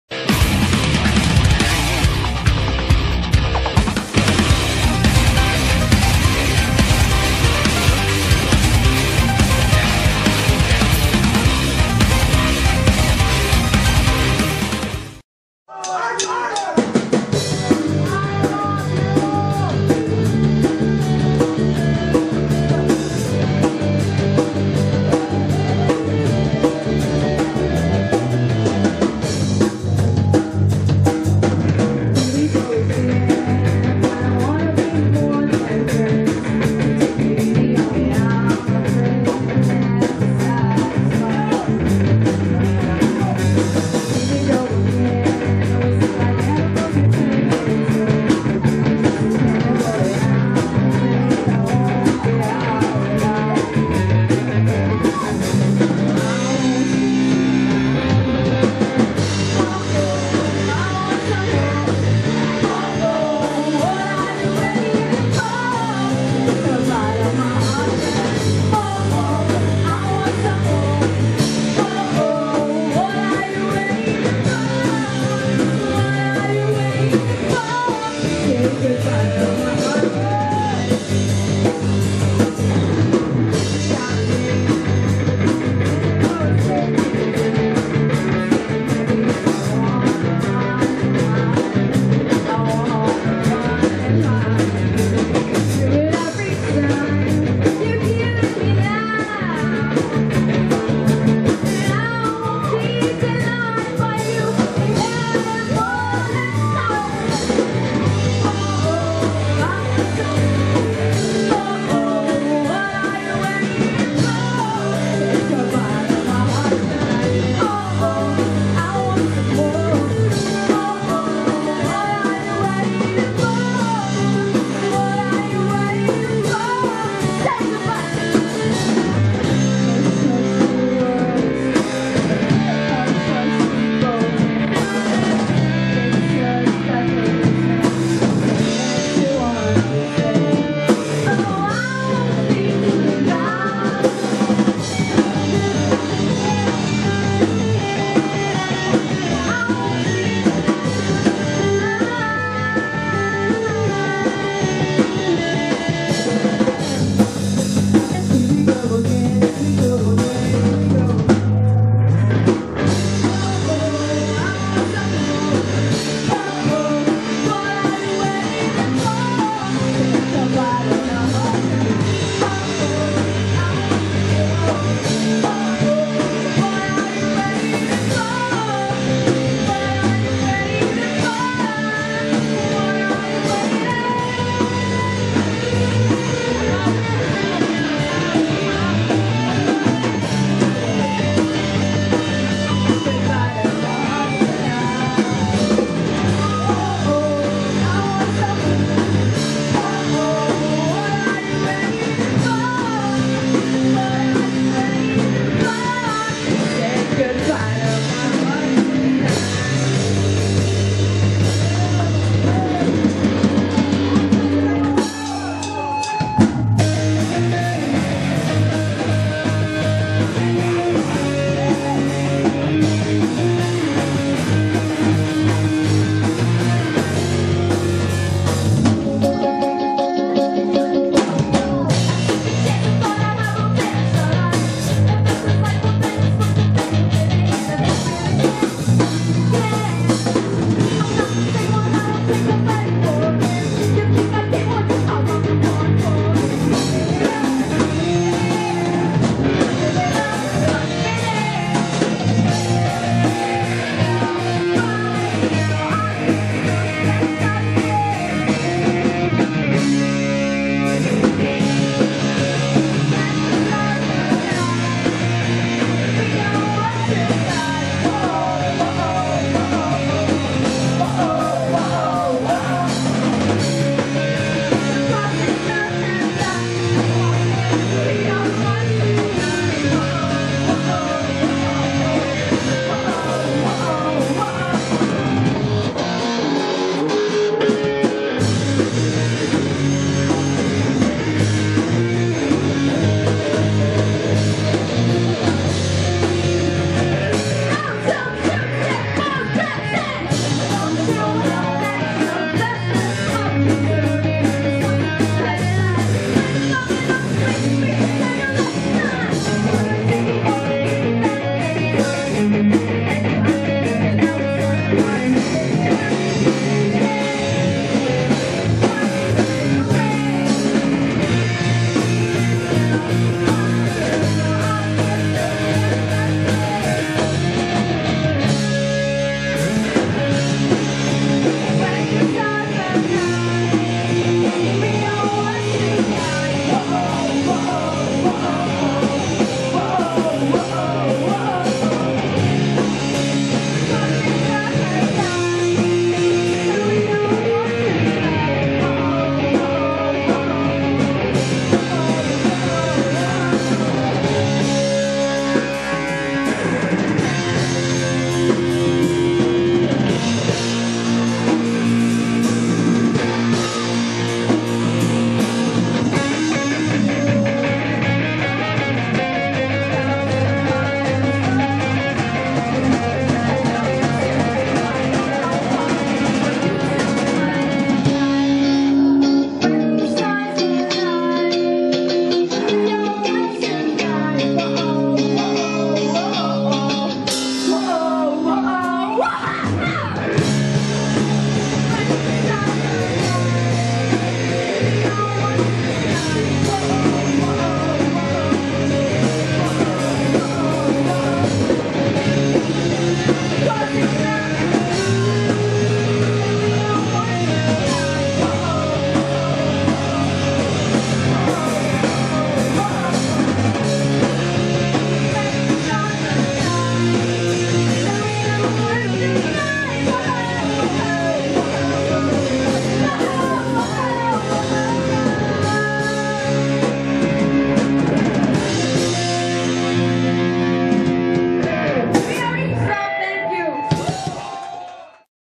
Eavesdrop live @ Kampay Resto Bar - Battleground 2015
Eavesdrop @ Kadena Production's "BATTLEGROUND 2015" @ Kampay Resto Bar. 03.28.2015